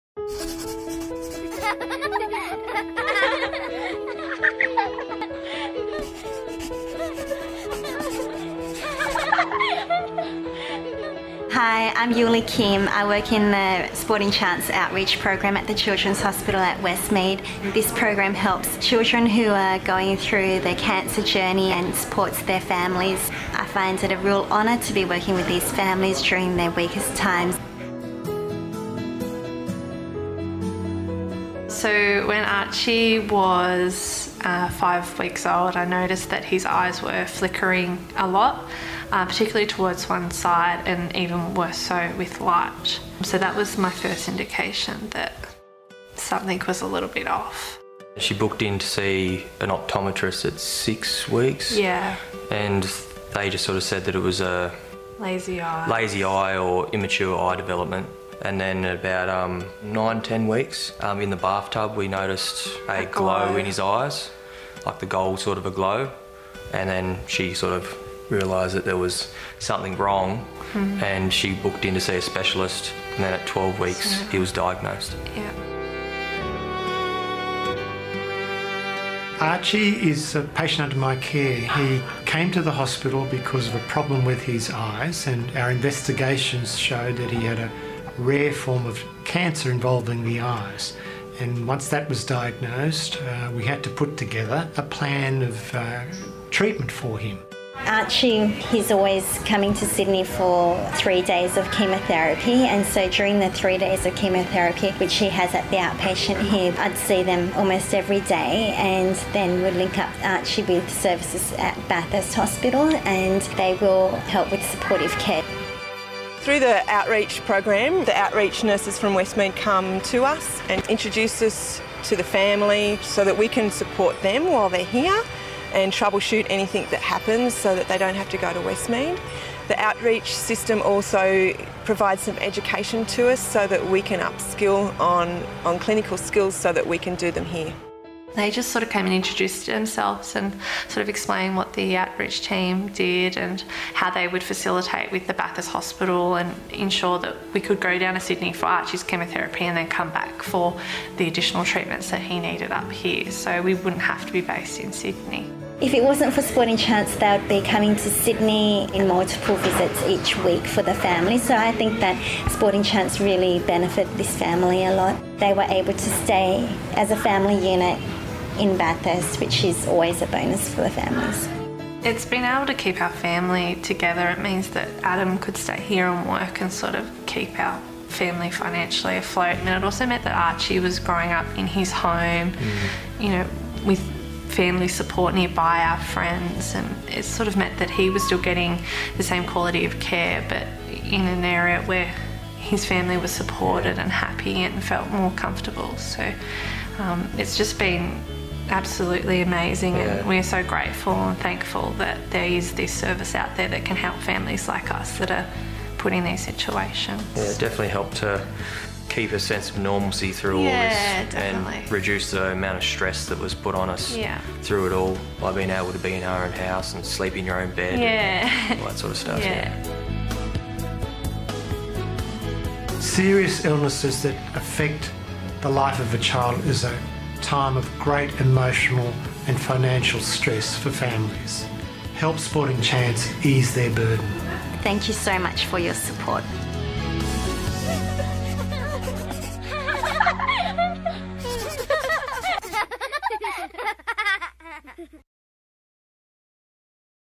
Sporting Chance Cancer Foundation - Archie's Story
At just five weeks old Archie’s mum started to notice irregular eye movement in her child. At 12 weeks he was diagnosed with a rare form of cancer of the eyes. The family lived in Bathurst, yet needed to attend regular treatment at The Children’s Hospital, Westmead. With support from the Sporting Chance Mark Taylor Outreach Program, Archie’s family was connected with Bathurst hospital. Assistance from the local hospital meant Archie’s family didn’t need to relocate to Sydney, or be separated for long periods during treatment. This enabled their life to be as normal as it could be under the circumstances, surrounded by family and friends.